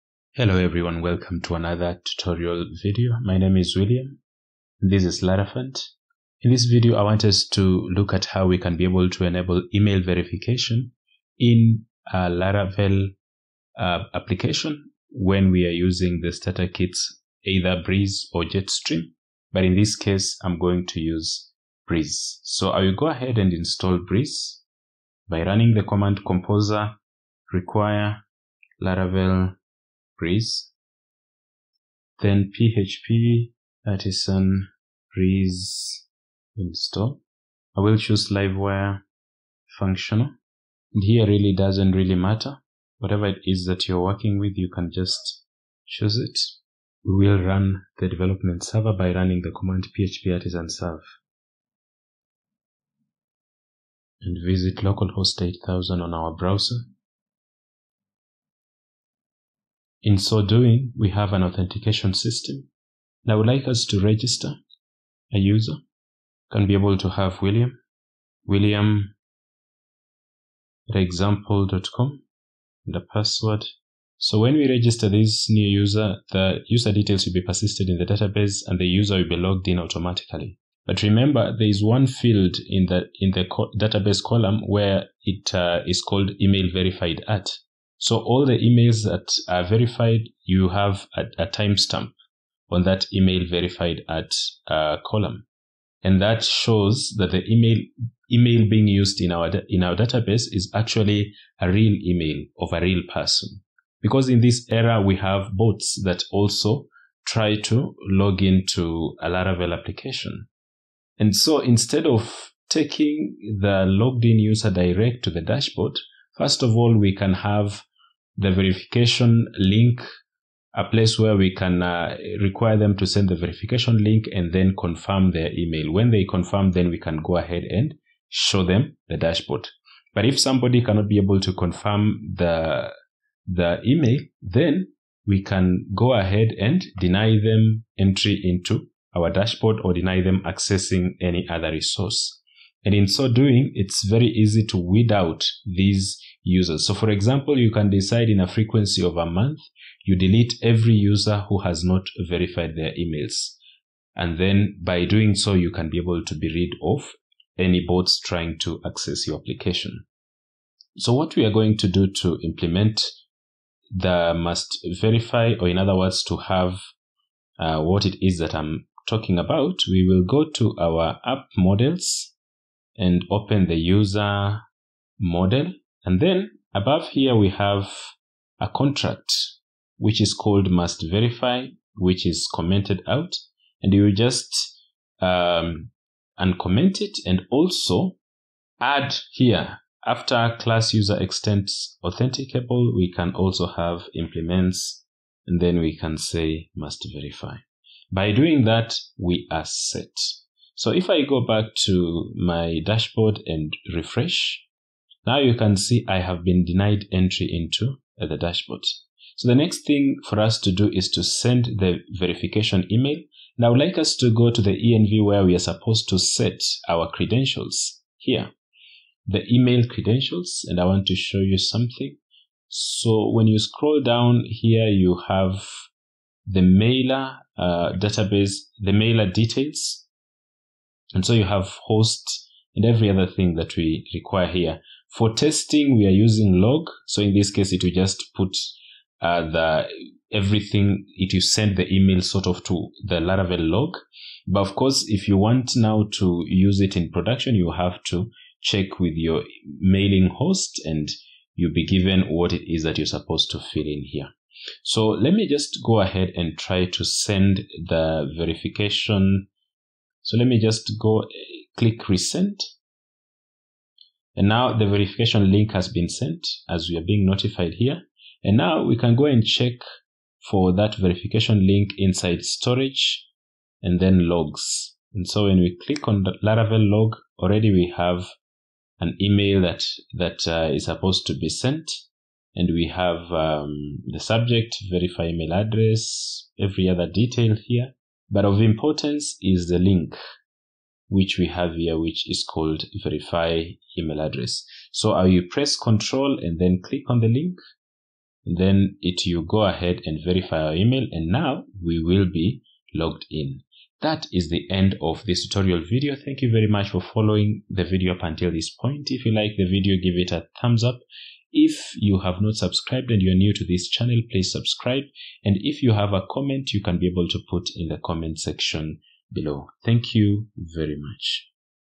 How to send Email Verification in Laravel 11
In this video tutorial we learn how to send verification email in Laravel 11